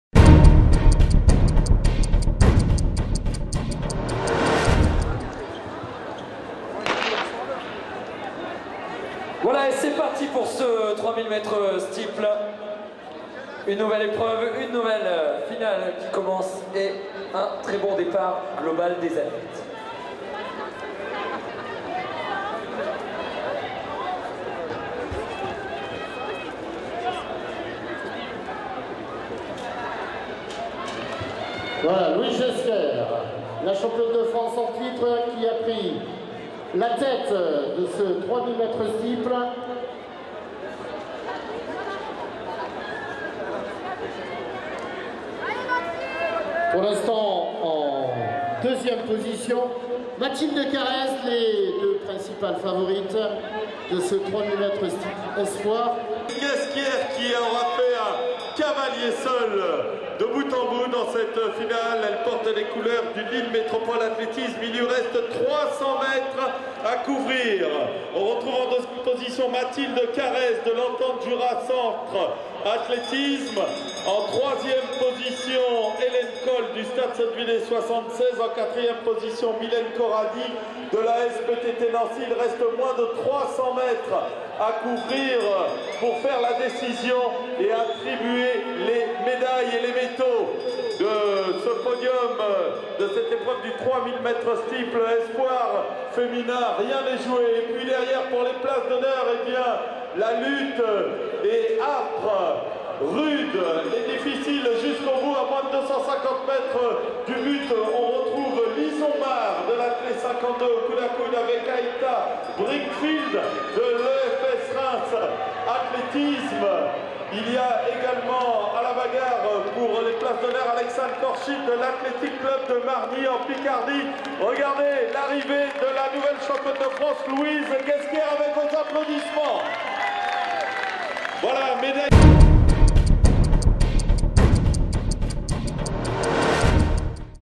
Bondoufle 10 juillet 2009 3000M steeple femmes Frances espoirs
1 10'49''00 GHESQUIERE Louise Lille Metropole Athletisme* 059 NPC ESF/88 N3 1020
2 10'54''81 CARREZ Mathilde Entente Jura Centre Athletisme 039 F-C ESF/89 N3 1007
3 10'59''89 COLLE Helene Stade Sottevillais 76* 076 H-N ESF/87 N3 995
4 11'05''91 CORRADI Mylene Asptt Nancy * 054 LOR ESF/89 R1 981
5 11'16''15 BRINKFIELD Heiata Efs Reims A. * 051 CHA ESF/88 R1 958
6 11'16''92 MARE Lison Athle 52 * 052 CHA ESF/87 R1 956
7 11'24''74 KORCHIT Alexane Athletic Club De Margny Les Co 060 PIC ESF/88 R1 939
8 11'36''77 NOBIS Mylene Manche Athle Centre Sud* 050 B-N ESF/89 R1 912
9 11'40''81 KARLESKIND Celia Athle St Julien 74 074 R-A ESF/87 R2 904
10 11'42''59 VAILLANT Marine Ac Villeneuve D'ascq 059 NPC ESF/88 R2 900
11 11'52''08 ROQUELAURE Cynthie Tremblay Ac Athletisme 093 I-F ESF/87 R2 879
12 11'58''27 VITTET Anne-sophie Lyon Athletisme* 069 R-A ESF/88 R2 866
13 12'06''91 DRAPEAU Aline Athletic Sud Vendee * 085 P-L ESF/89 R2 848
14 12'14''30 BOUTEILLE Charlotte Nice Cote D'azur Athletisme * 006 C-A ESF/89 R2 832